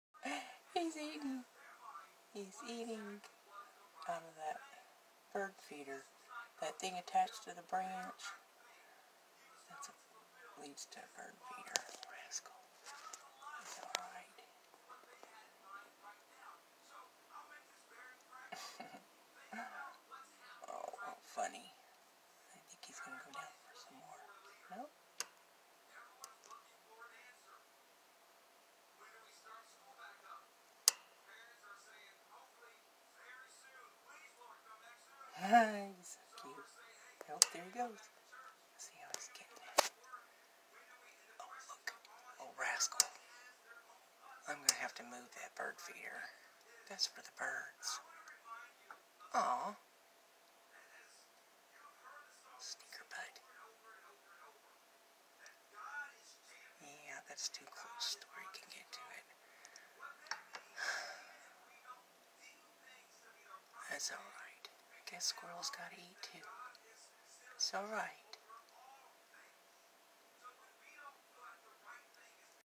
BANDIT THE SQUIRREL ROBBING THE BIRD FEEDER
Cute lil critter getting its share of bird feed...well moved the feeder on top of a feeding station that no squirrels can get on top of. Though I do pay their extortion fee to not bother the bird eggs in nest of trees nearby so they still get their share but the birds can eat in peace.